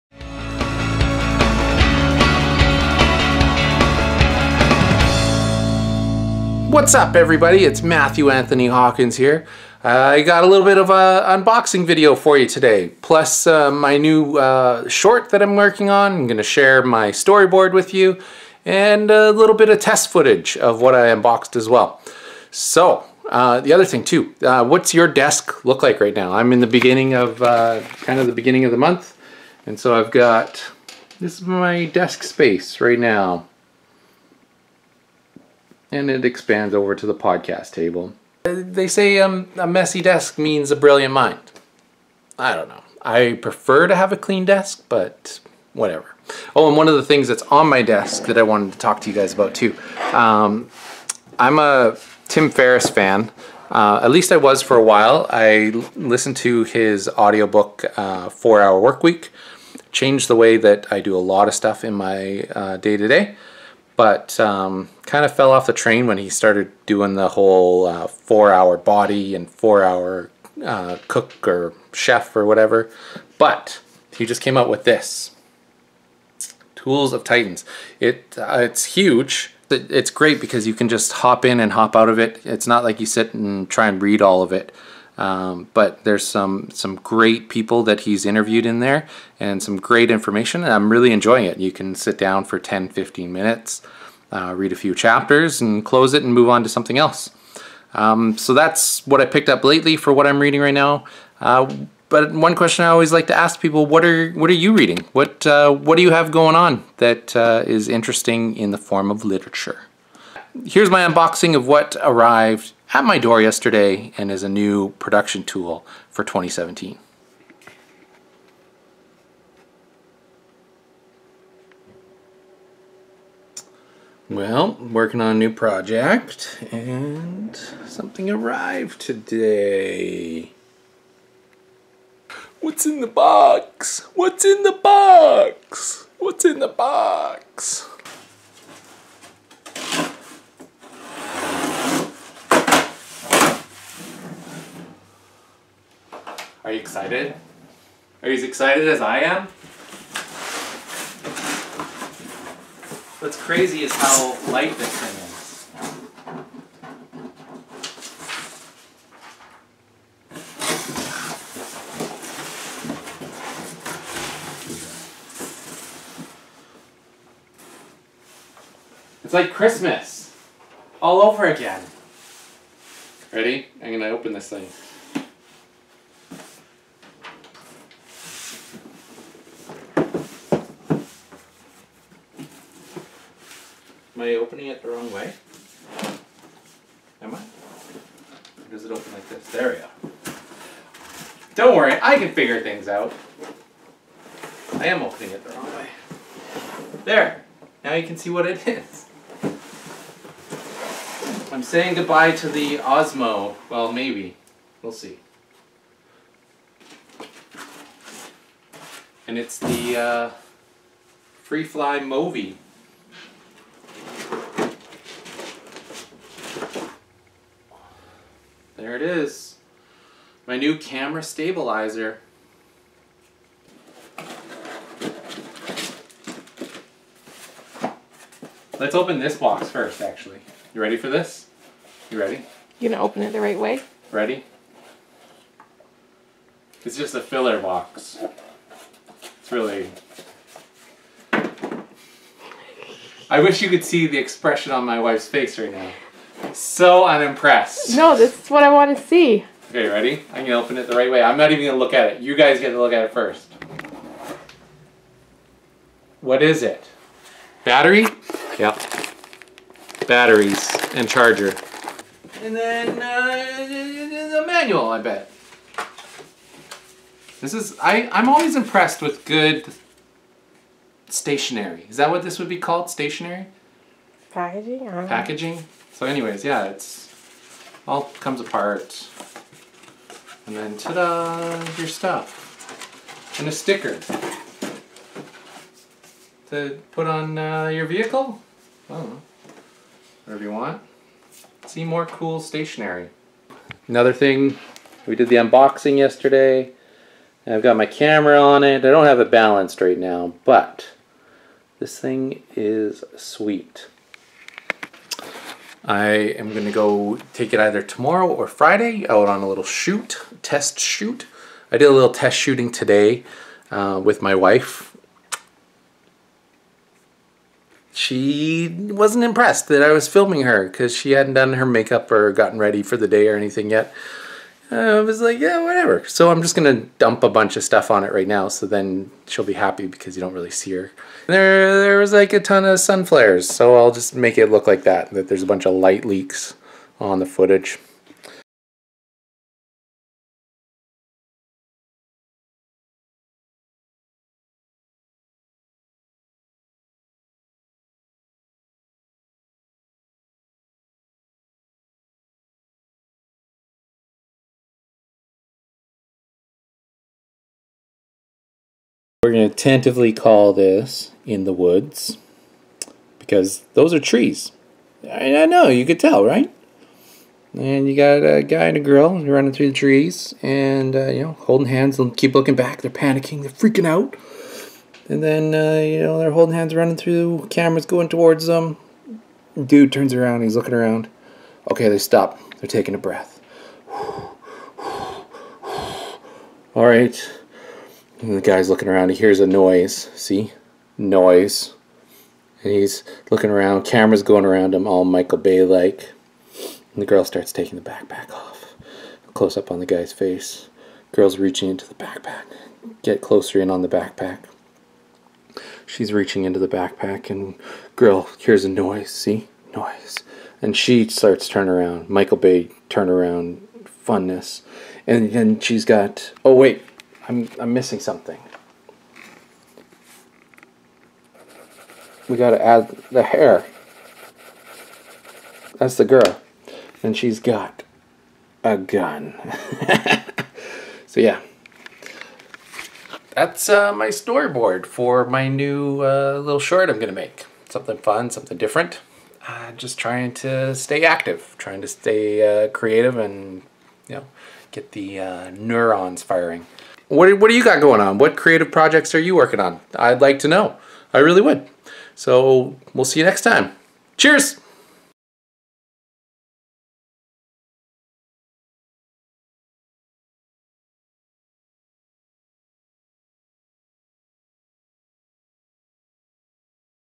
Unbox Freefly Movi M5 | Camera Gear I Use
I recently purchased the Freefly Movi M5 to use in my shoots. I do a little unboxing of when it arrived.

Plus I share a page from my new storyboard for a short I am looking forward to filming very shortly.

Had a couple questions for you this episode too..
What creative works are you doing right now?
What are reading right now?
What does your desk look like right now?